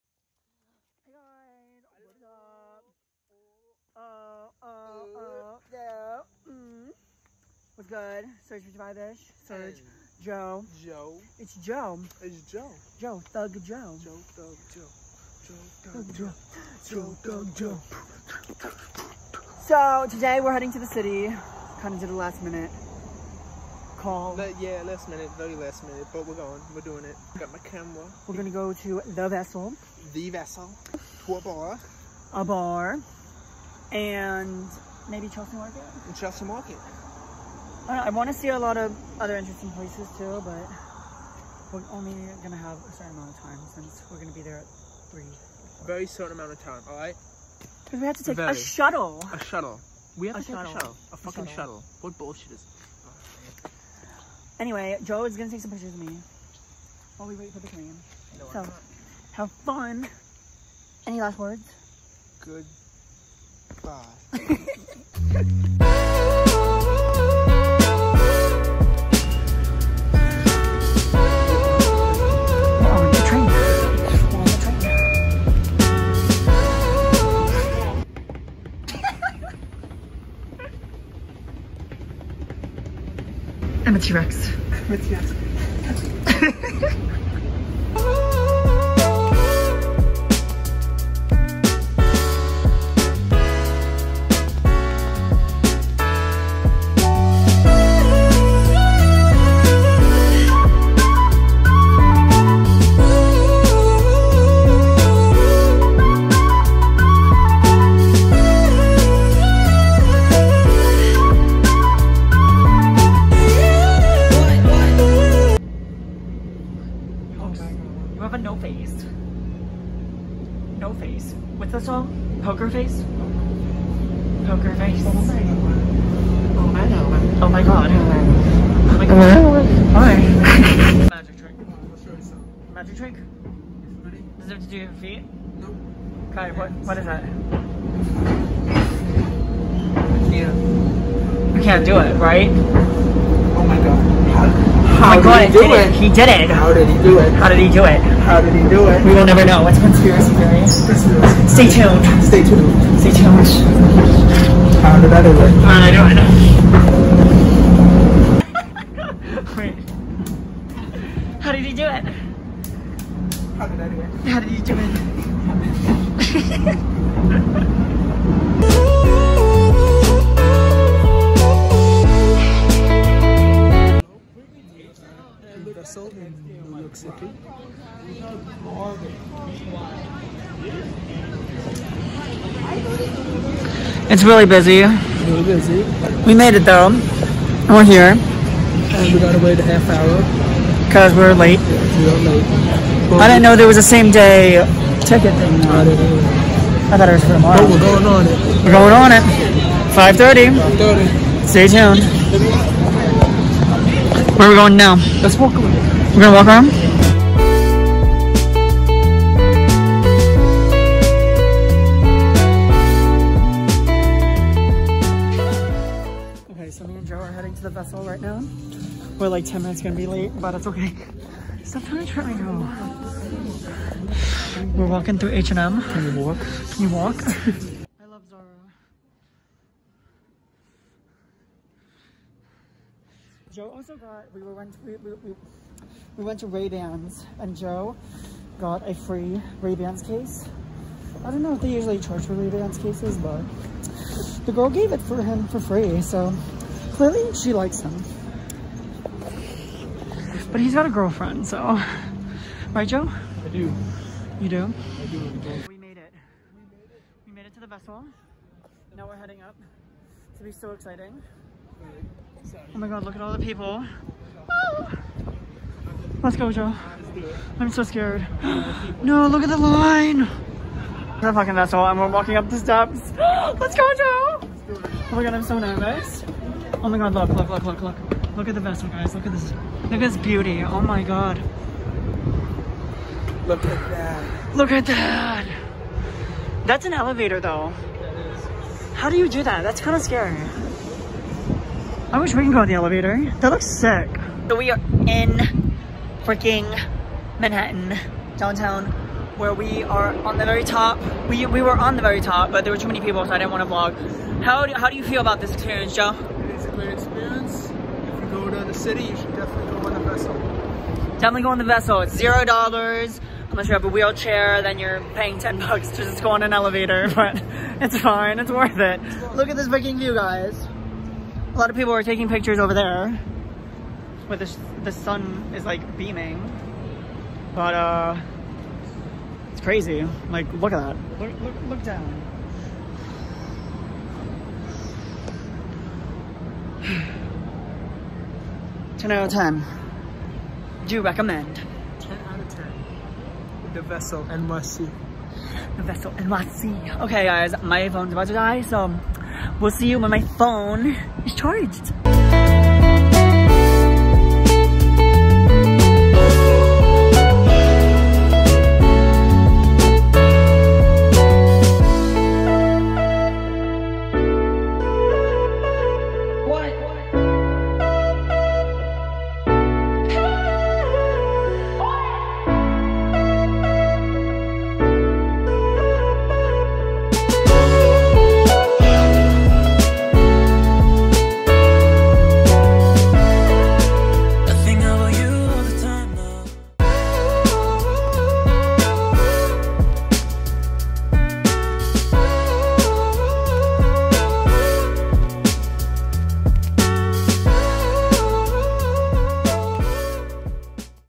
THE VESSEL W/ JOE! | VLOG | SERG55ISH
Went into the city with my good friend joe enjoy our craziness!